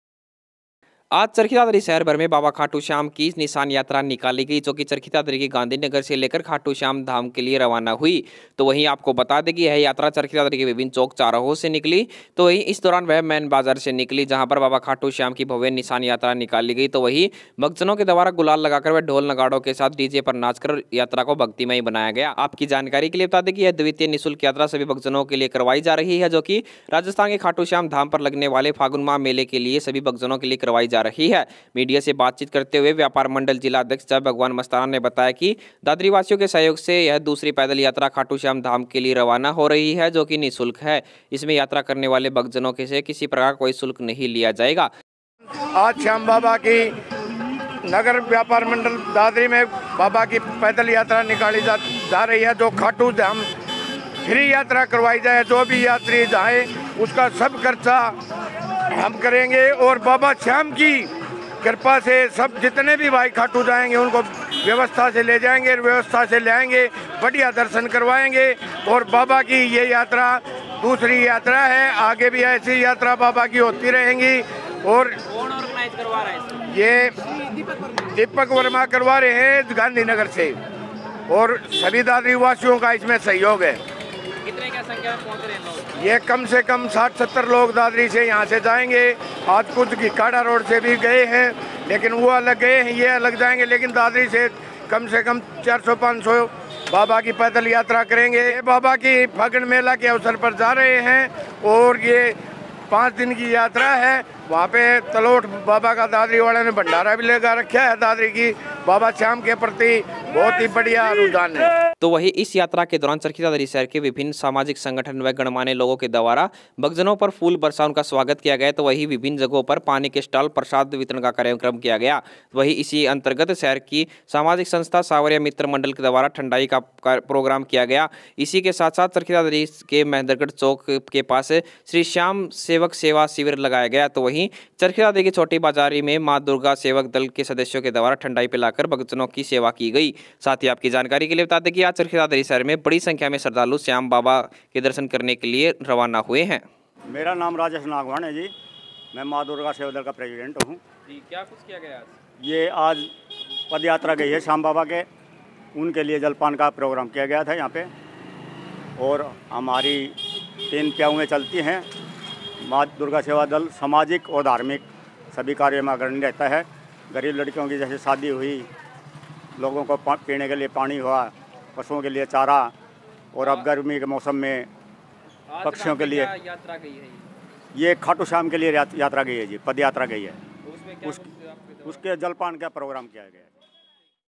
खाटू श्याम धाम के लिए निकली चरखी दादरी से पदयात्रा || श्रद्धालु हुए शामिल || VTS LIVE NEWS
Haryana News on just a click.
एक क्लिक पर जुड़िए हरियाणा की छोटी बड़ी खबरों से जिनसे आप अब तक थे दूर...अब खबरें आप तक पहुचायेंगें हम...